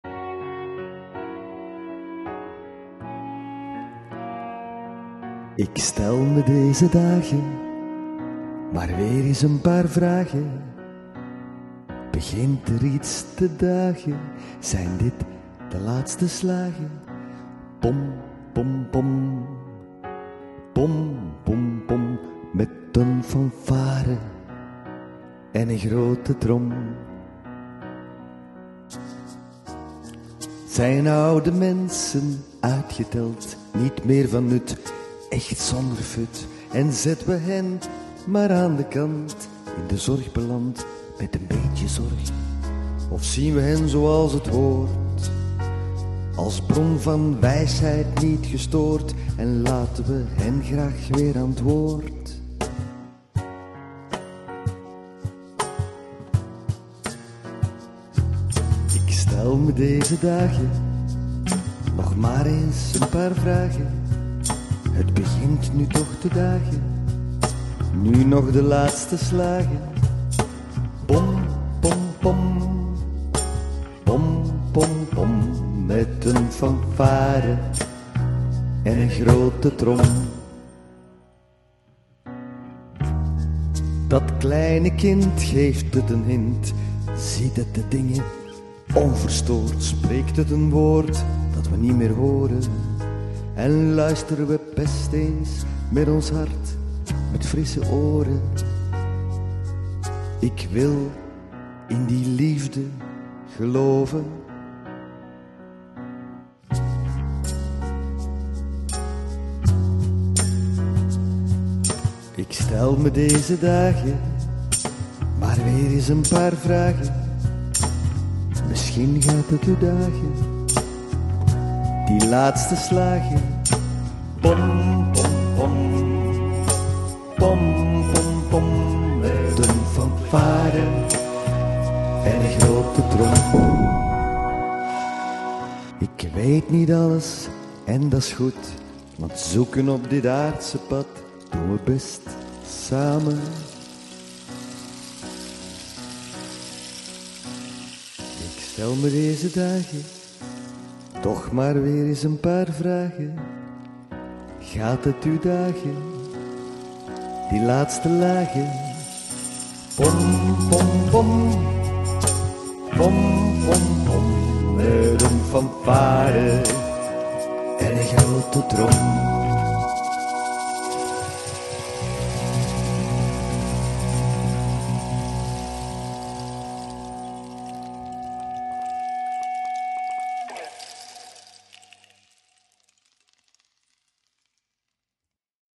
2024 Wensen Klankmakerij Lucaz
Nieuwjaarsfilm Klankmakerij Lucaz, thanks for images to Els Loos, Ann O, Bieke,...
Artwork Veerle Vaessens
first recording with real studio equipment after burn out and many recordings on iPad and iPhone.
Bass, cachon, shaker, tamboerijn, rainstick, Guitar, keyboards and vocals by Lucaz
listen also on Spotify